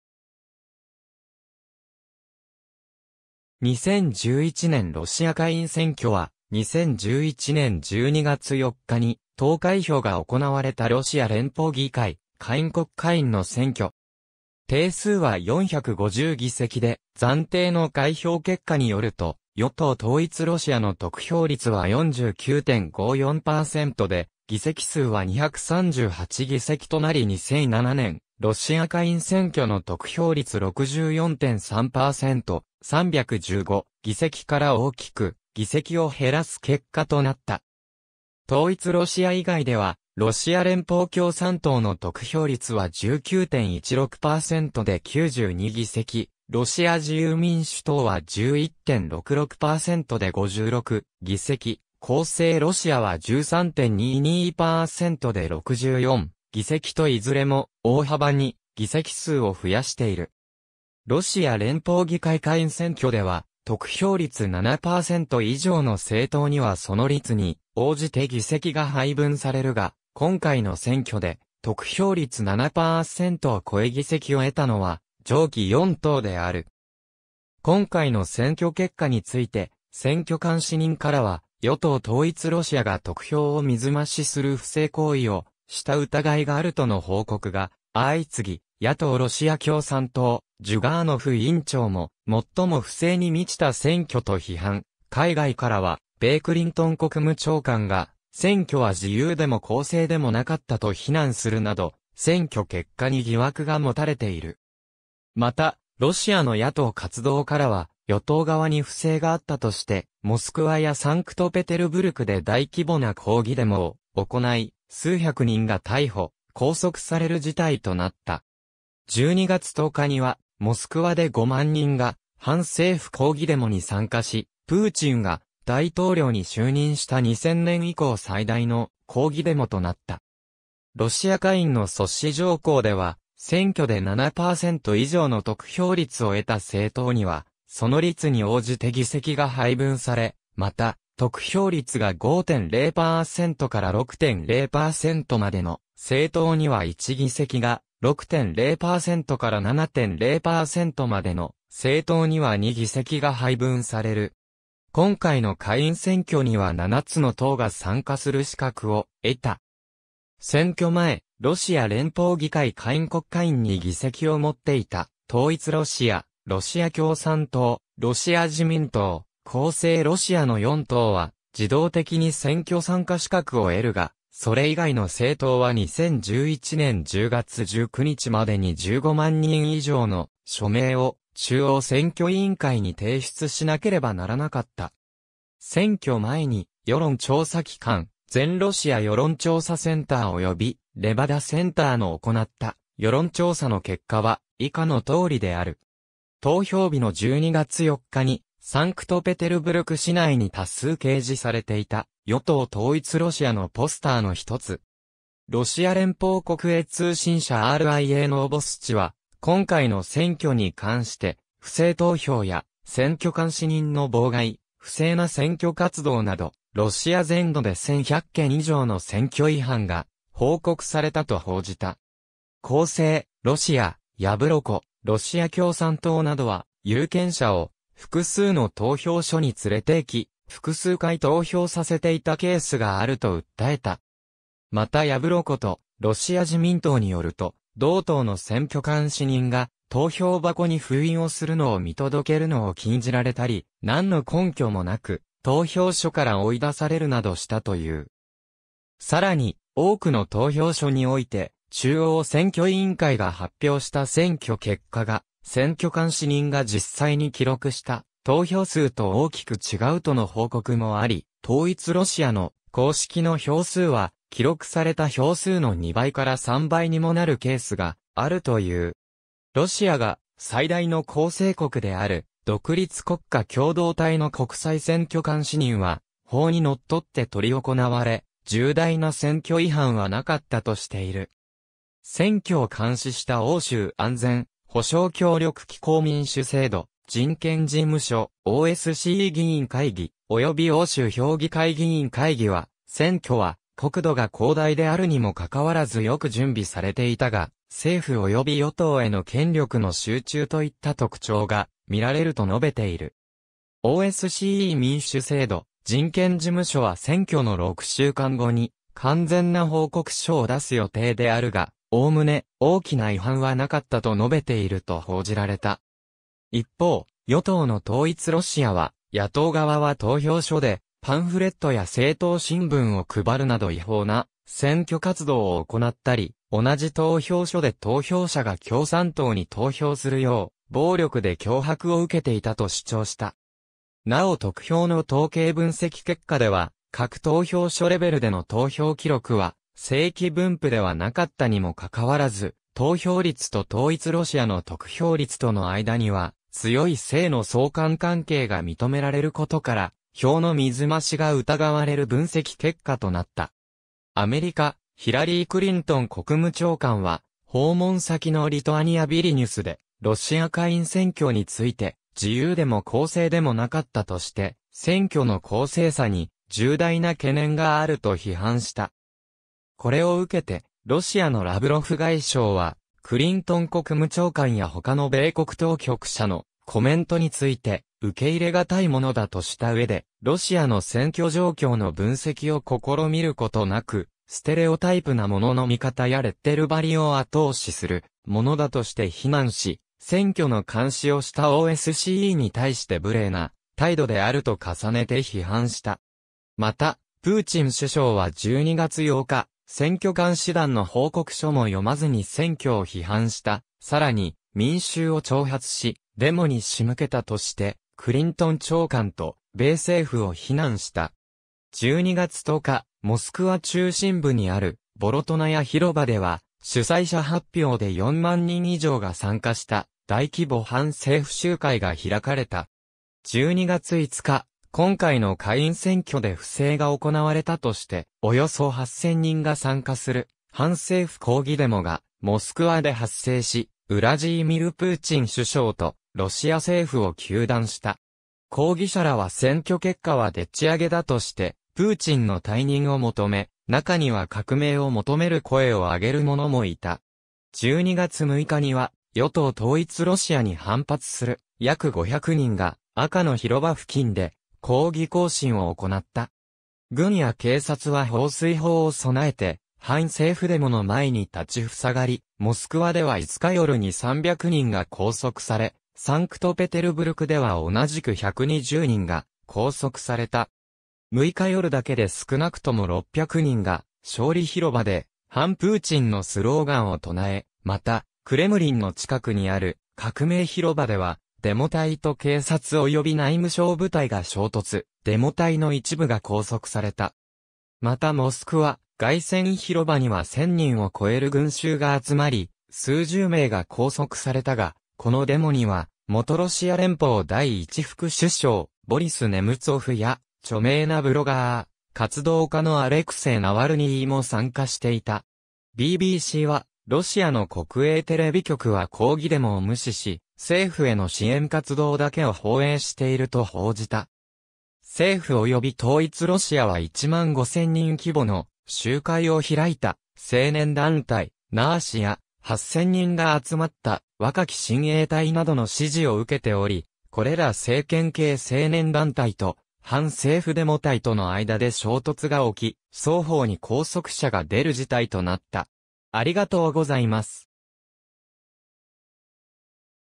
2011年ロシア下院選挙
ドミートリー・メドヴェージェフ
2011年ロシア下院選挙（2011ねんロシアかいんせんきょ）は、2011年12月4日に投開票が行われたロシア連邦議会下院国家院の選挙。
定数は450議席で、暫定の開票結果によると与党統一ロシアの得票率は49.54％で議席数は238議席となり2007年ロシア下院選挙の得票率64.3％、315議席 から大きく議席を減らす結果となった。
統一ロシア以外では、ロシア連邦共産党の得票率は19.16％で92議席、ロシア自由民主党は11.66％で56議席、公正ロシアは13.22％で64議席といずれも大幅に議席数を増やしている。
ロシア連邦議会下院選挙では、得票率7％以上の政党にはその率に応じて議席が配分されるが、今回の選挙で得票率7％を超え議席を得たのは上記4党である。
今回の選挙結果について、選挙監視人からは与党統一ロシアが得票を水増しする不正行為をした疑いがあるとの報告が相次ぎ、野党ロシア共産党ジュガーノフ委員長も「最も不正に満ちた選挙」と批判、海外からは米クリントン国務長官が「選挙は自由でも公正でもなかった」と非難するなど、選挙結果に疑惑がもたれている。
また、ロシアの野党活動家らは与党側に不正があったとしてモスクワやサンクトペテルブルクで大規模な抗議デモを行い、数百人が逮捕・拘束される事態となった。
12月10日にはモスクワで5万人が反政府抗議デモに参加し、プーチンが大統領に就任した2000年以降最大の抗議デモとなった（2011年ロシア反政府運動）。
ロシア下院の阻止条項（足切り条項）では、選挙で7％以上の得票率を得た政党にはその率に応じて議席が配分され、また得票率が5.0％から6.0％までの政党には1議席が、6.0％から7.0％までの政党には2議席が配分される。
今回の下院選挙には7つの党が参加する資格を得た。
選挙前ロシア連邦議会下院国家院に議席を持っていた統一ロシア、ロシア共産党、ロシア自民党、公正ロシアの4党は自動的に選挙参加資格を得るが、それ以外の政党は2011年10月19日までに15万人以上の署名（ただし1地域につき最大5,000人までの署名しか認められない）を中央選挙委員会に提出しなければならなかった。
選挙前に世論調査期間「全ロシア世論調査センター (VTSIOM) （VCIOM）」および「レバダ・センター (Levada Center) 」の行った世論調査の結果は以下の通りである。
投票日の12月4日にサンクトペテルブルク市内に多数掲示されていた与党統一ロシアのポスターのひとつ。
ロシア連邦国営通信社RIAノーボスチは、今回の選挙に関して不正投票や選挙監視人の妨害、不正な選挙活動など、ロシア全土で1,100件以上の選挙違反が報告されたと報じた。
公正ロシア、ヤブロコ、ロシア共産党などは、有権者を複数の投票所に連れて行き複数回投票させていたケースがあると訴えた。
またヤブロコとロシア自民党によると、同党の選挙監視人が投票箱に封印をするのを見届けるのを禁じられたり、何の根拠もなく投票所から追い出される（これはビデオ映像も残っている）などしたという。
さらに、多くの投票所において中央選挙委員会が発表した選挙結果が、選挙監視人が実際に記録した投票数と大きく違うとの報告もあり、統一ロシアの“公式の”票数は、記録された票数の2倍から3倍にもなるケースがあるという。
ロシアが最大の構成国である独立国家共同体の国際選挙監視人は「（今回の選挙は）法に則って執り行われ、重大な選挙違反はなかった」としている。
選挙を監視した欧州安全保障協力機構（OSCE）民主制度・人権事務所、OSCE議員会議、および欧州評議会議員会議（PACE）は、選挙は国土が広大であるにもかかわらずよく準備されていたが、政府及び与党への権力の集中といった特徴がみられると述べている。
OSCE民主制度･人権事務所は選挙の6週間後に完全な報告書を出す予定であるが、概ね「大きな違反はなかった」と述べていると報じられた。
一方、与党の統一ロシアは、野党側は投票所でパンフレットや政党新聞を配るなど違法な選挙活動を行ったり、同じ投票所で投票者が共産党に投票するよう暴力で脅迫を受けていたと主張した。
なお得票の統計分析結果では、各投票所レベルでの投票記録は正規分布（平均値の付近に集積するようなデータの分布）ではなかったにもかかわらず、投票率と統一ロシアの得票率との間には強い正の相関関係（一方が増えれ...